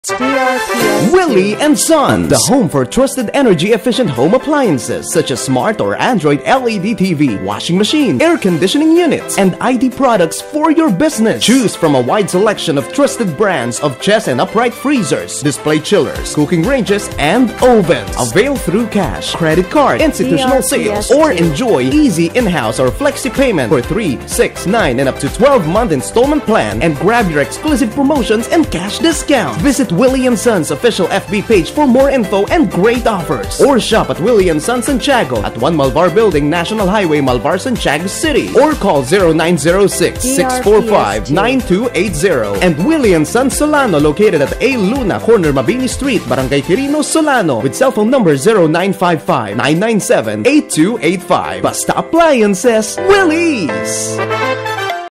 Willy and Sons Radio Ad Q2 2023 55s (Santiago, Isabela)
Ito po ang Q2 2023 Provincial Radio Ad ng Willy and Sons (a.k.a. Willy's) sa Santiago, Isabela.

DRPS Two 3821: Willy and Sons Radio Ad Q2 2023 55s (Santiago, Isabela)